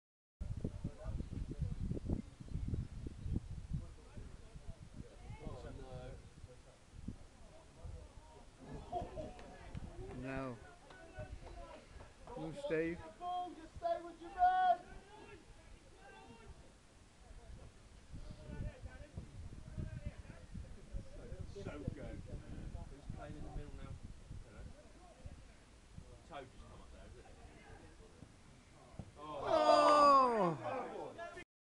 ECPL DIV 1 GAME ,ST STEPHEN 1st 1 - 0 St Teath
A Steve Raven save then St Stephen have a chance but Courtney Rowe fires high and wide.